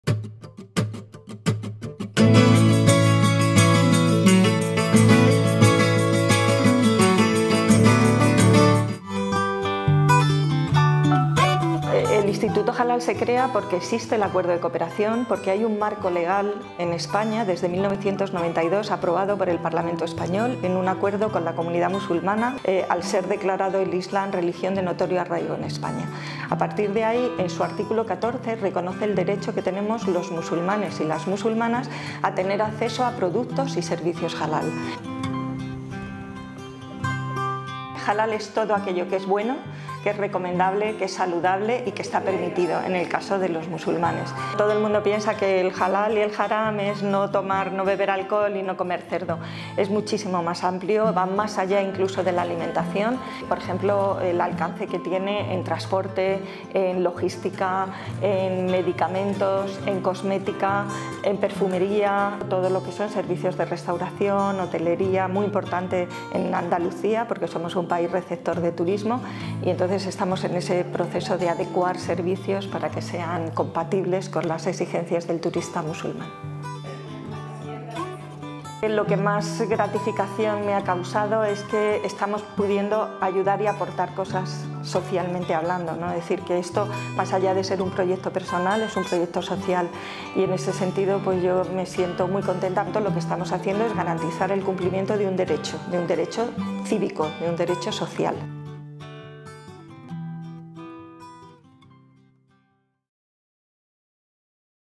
Isabel Romero, un sello de garantía halal para los productos destinados al público musulmán
Isabel Romero es la directora general del Instituto Halal, la entidad que desde 1996 se encarga de certificar en España los productos y servicios que son aptos para el consumo de los musulmanes. Mucho más allá del ámbito de la alimentación, ya hay más de un millar de productos certificados y 340 empresas que siguen las normas del islam, garantía tanto para los musulmanes que viven en nuestro país como para el creciente mercado del turismo musulmán.